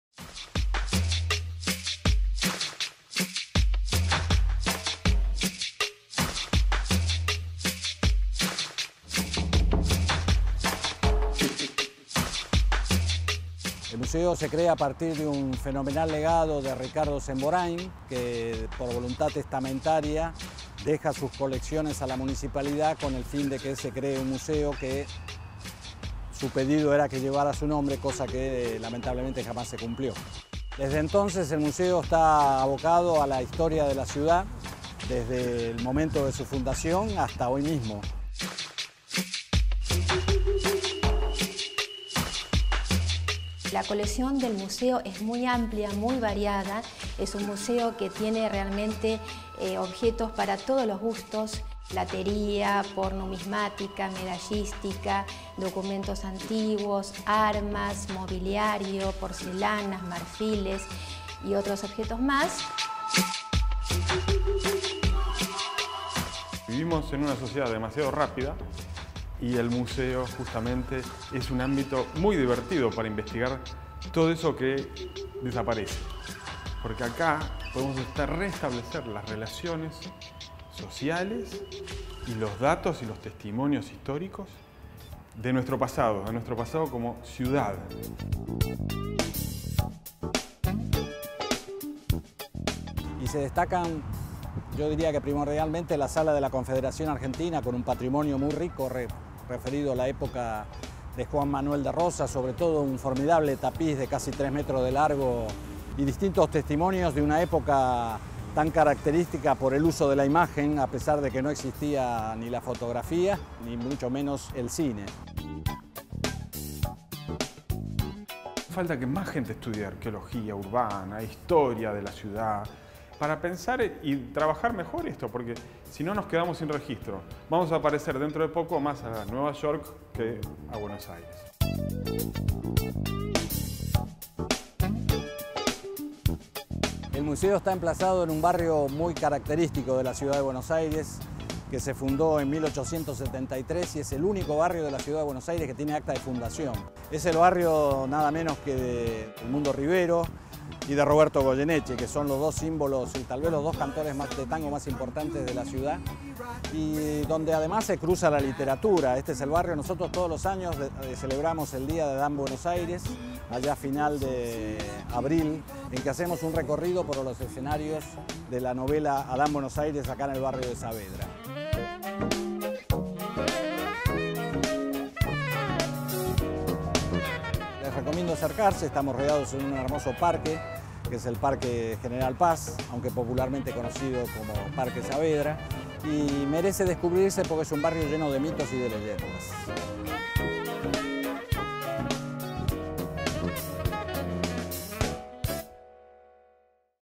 Para descubrir: Museo histórico de Buenos Aires, Cornelio Saavedra en Disfrutemos BA
En para descubrir visitamos el Museo histórico de Buenos Aires, Cornelio Saavedra (Av. Crisólogo Larralde 6309 - Comuna 12) en Disfrutemos BA.